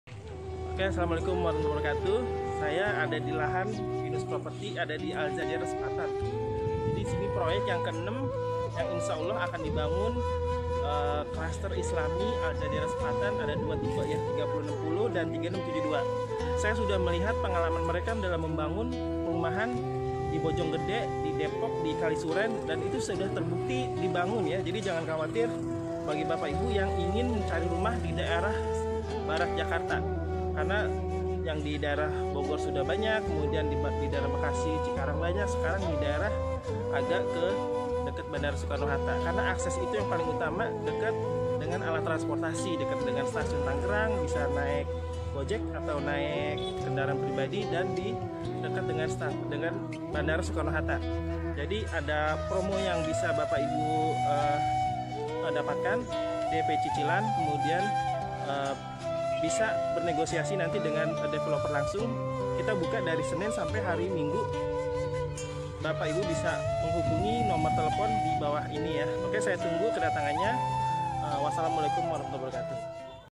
Kavling dan Cluster Syariah Al Jazeera Sepatan
Assalamualaikum...

Setelah Sukses di 4 project sebelumnya perumahan dengan Konsep  lingkungan yg Syariah Di Bogor.

Kini Pinus Property Hadir untuk Anda  Di TANGERANG dekat ke Bandara Soekarno Hatta.

CLUSTER AL-JAZEERA SEPATAN
Tanah Merah, Sepatan Timur Tangerang

Unit Tersedia :
Rumah Type 30/60
Rumah Type 36/72
Ruko Komersil uk. 5 X 10

Lokasi Sangat Strategis karena hanya :
4 Km ke Bandara Soekarno Hatta
km ke SDN Tanah Merag
km ke SMPN 1 SEPATAN
Km ke SMAN 11 SEPATAN

Pembayaran dengan Skema syariah
Tanpa Bunga, Tanpa Denda, Tanpa Sita, Tanpa Bank, BEBAS RIBA

"Ayo segera Survey dan Dapatkan Harga Promo Launching nya!"

PERSYARATAN MUDAH TANPA BI CHEKING
LANGSUNG ACC DEVELOPER
Pedagang, Wiraswasta, karyawan, PNS dll

SEMUA BISA PUNYA RUMAH !!!

WA or Call di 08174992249 / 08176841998